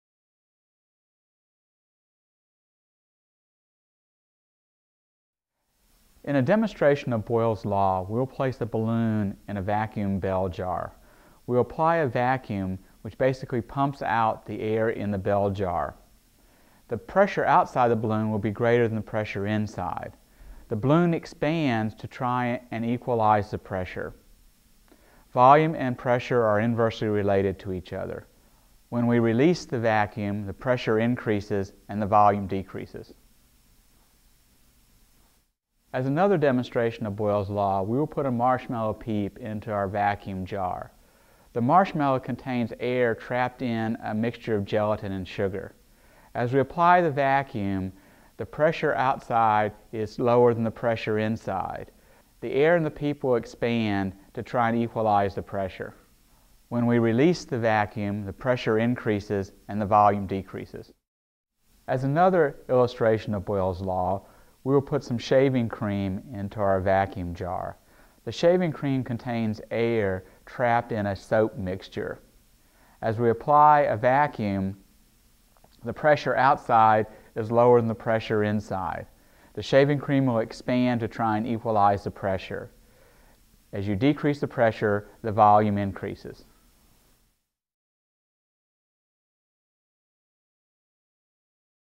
Demostration of Boyle's Law(Pressure And Volume)
Source Description :

Part of NCSSM CORE collection: This video shows the relationship between volume and pressure by placing a balloon, a marshmallow, and shaving cream a vacuum bell jar.

Please attribute this work as being created by the North Carolina School of Science and Mathematics.